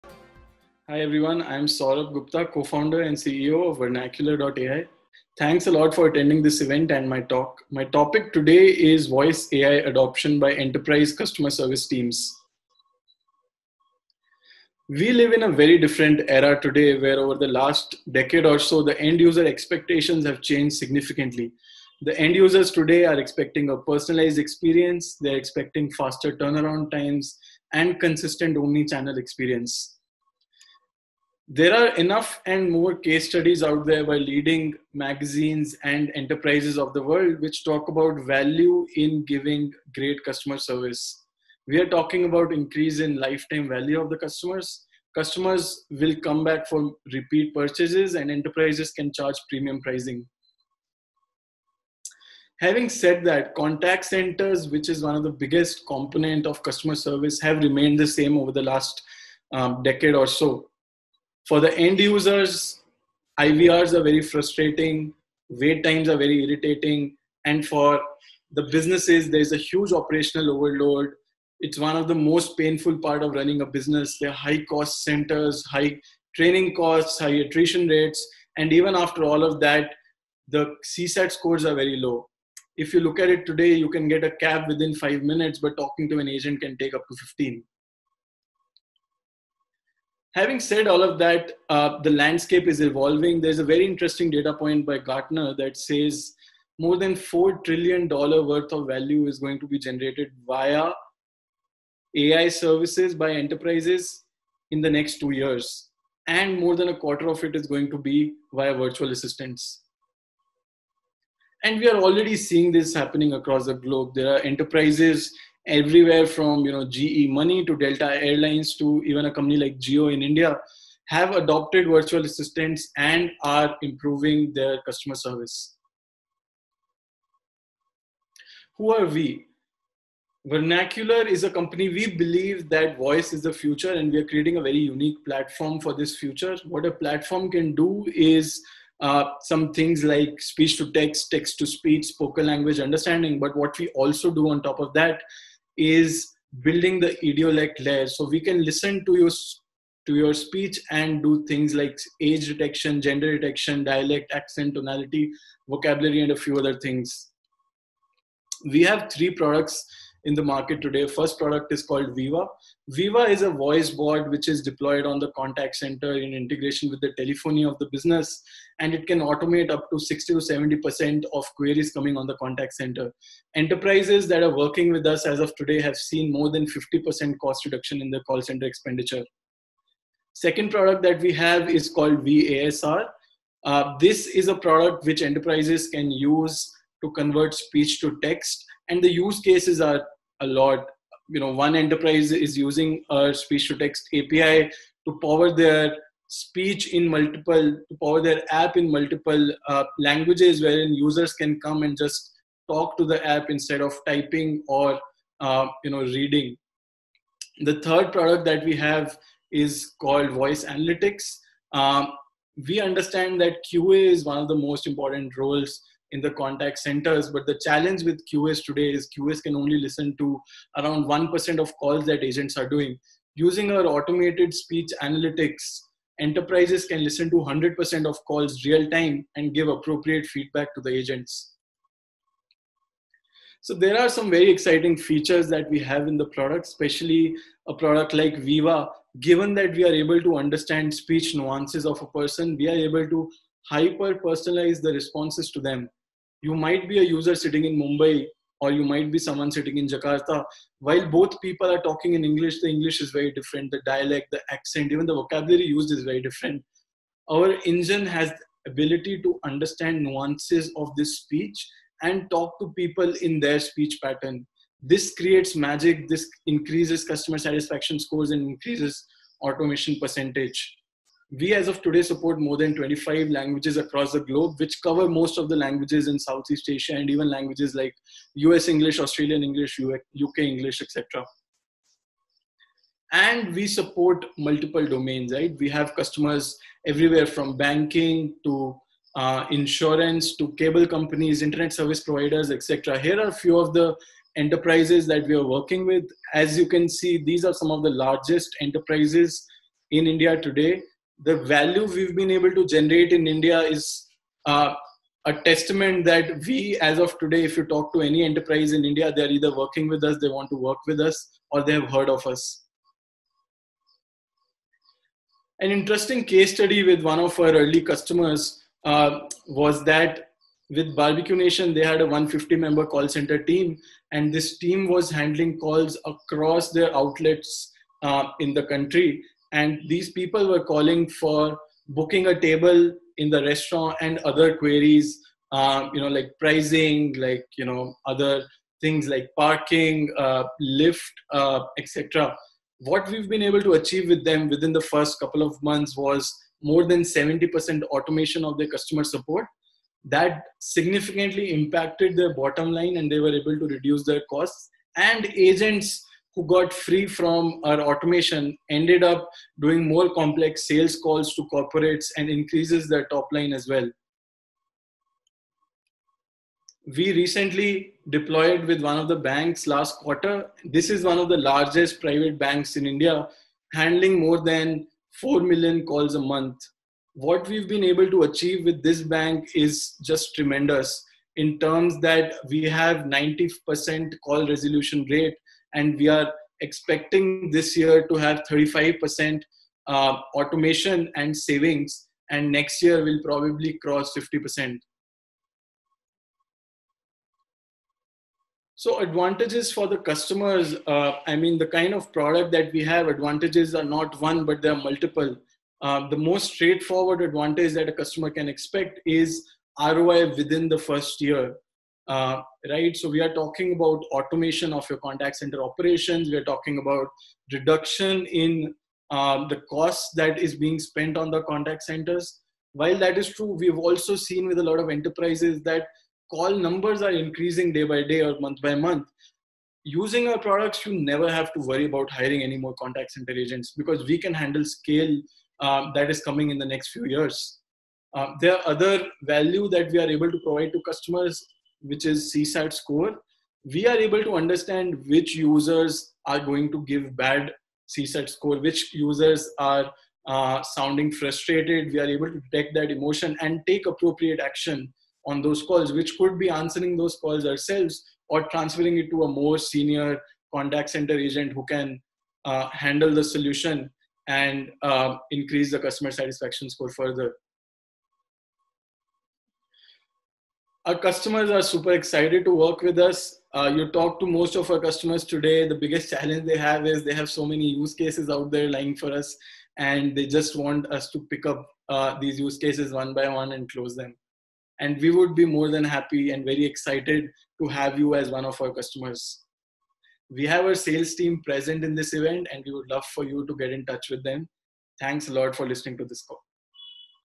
VOICE Global | 15 mins with Vernacular.ai
15 mins with Vernacular.ai

VOICE Global is a 24 hour livestream event that connects the global voice-tech industry and features thought leaders across development, design, startups, brands, agencies and major platforms